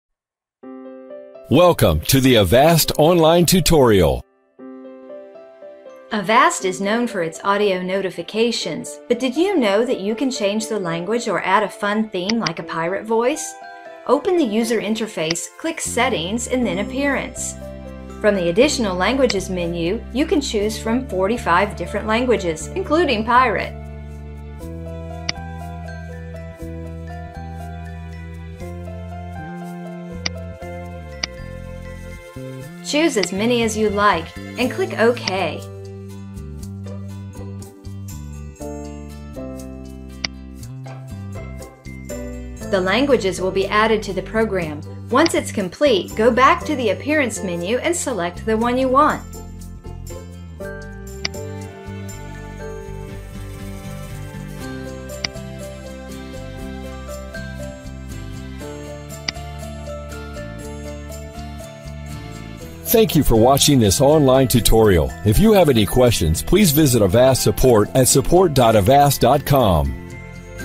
How to change the language in Avast Security products
Avast is known for it's audio notifications. The program has 45 different languages to choose from, plus some fun themes.

Need help with something?